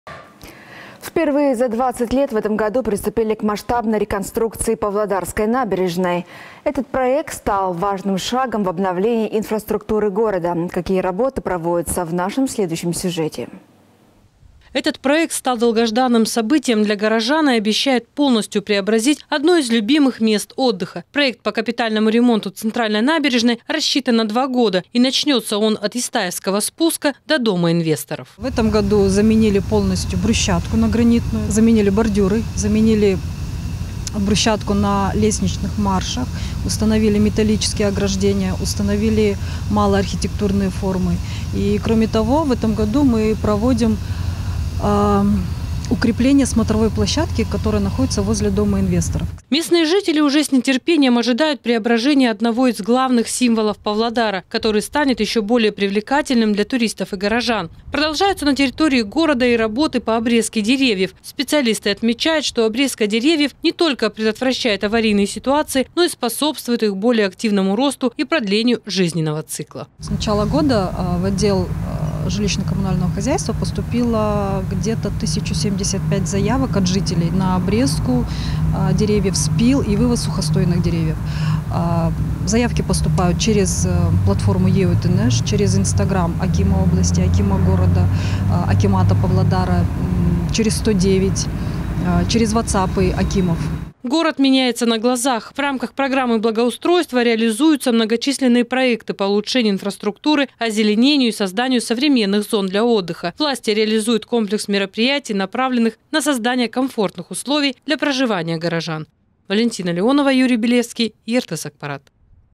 Масштабная работа
Впервые за 20 лет в этом году приступили к масштабной реконструкции павлодарской набережной. Этот проект стал важным шагом в обновлении инфраструктуры города. Какие работы проводятся – в нашем следующем сюжете.